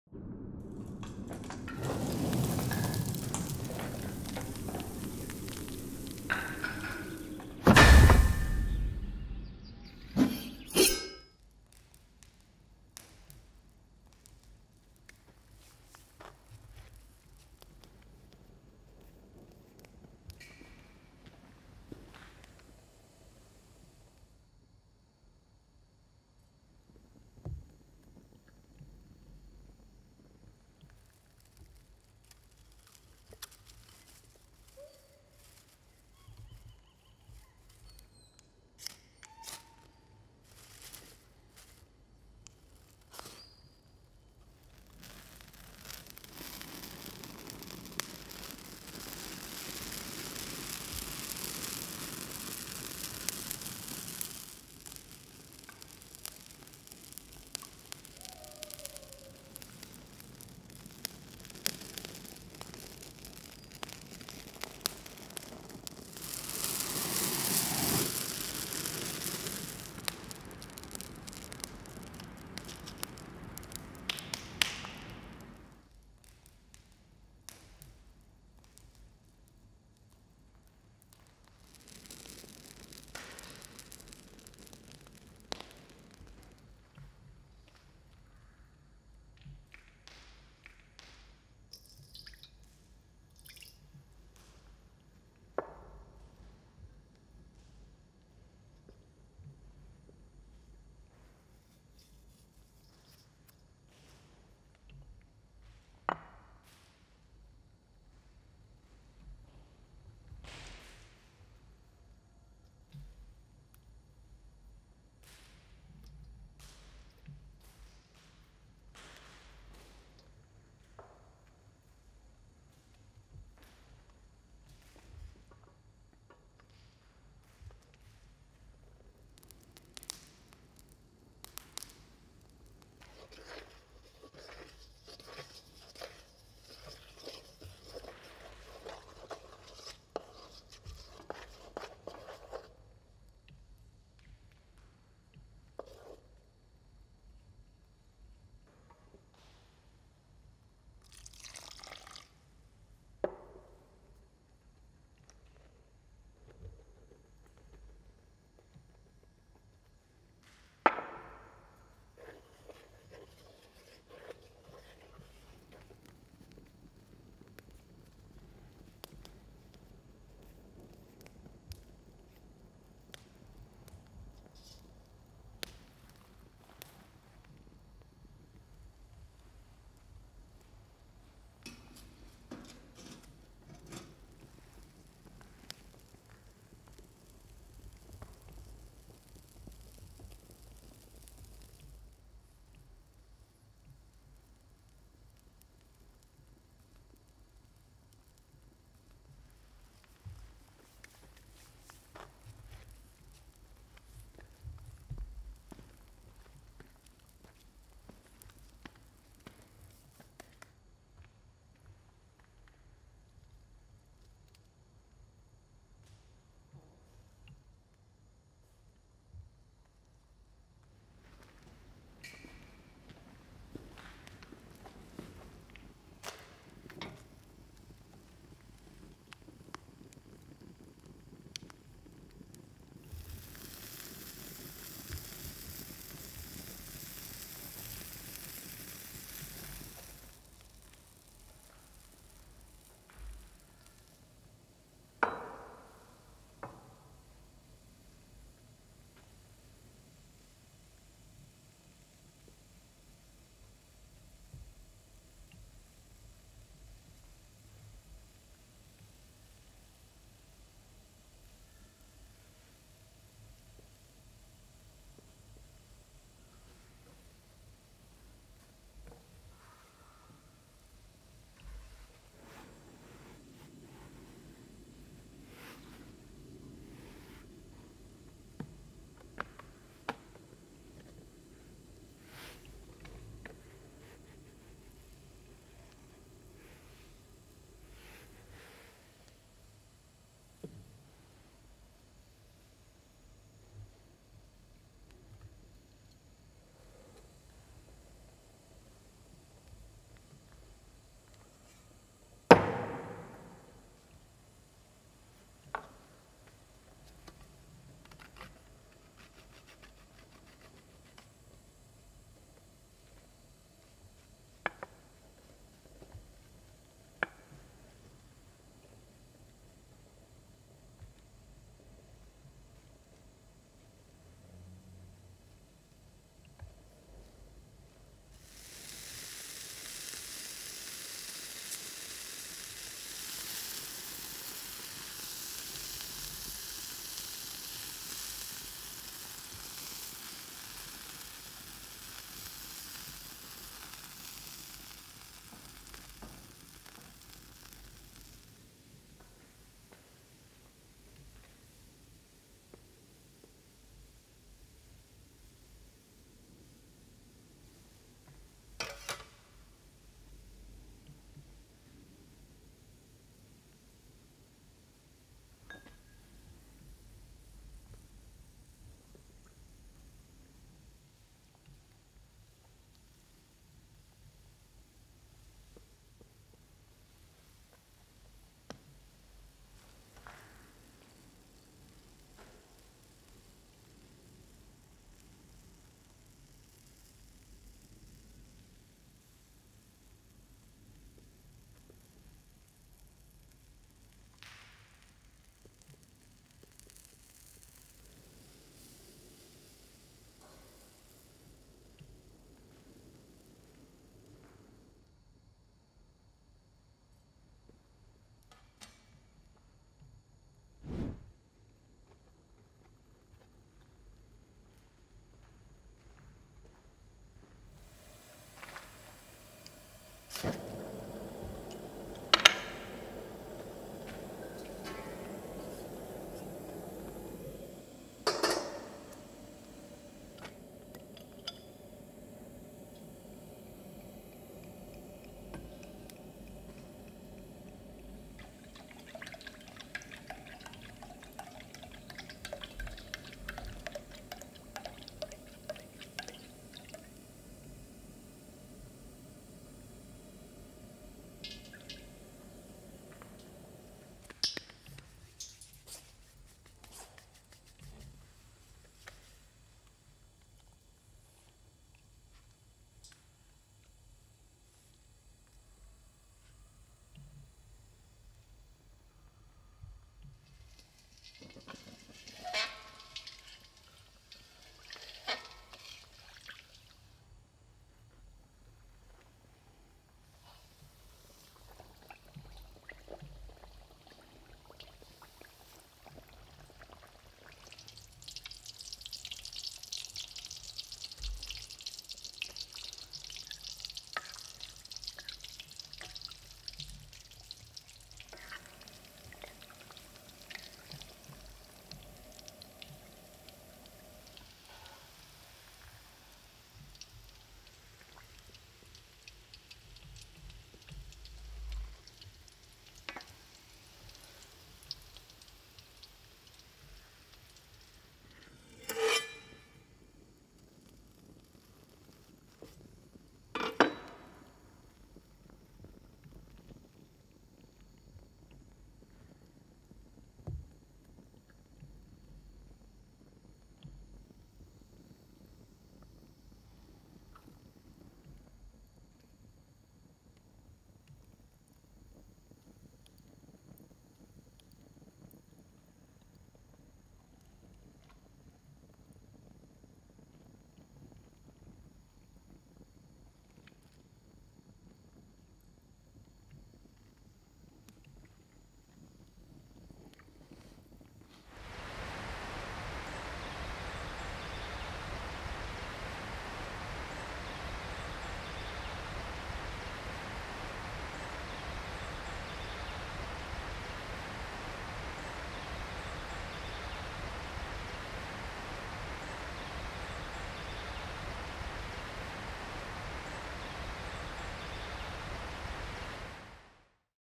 Scrumptious Hot Dog Recipe | Cyberpunk Food
Today's recipe is for deep-fried hot dogs that are sure to satisfy your cravings! To begin, grill the hot dogs for a short time before wrapping them up like a mummy in a green blanket filled with plenty of grated cheese. This dish can be made over an open fire or in a regular oven. Bon appetit!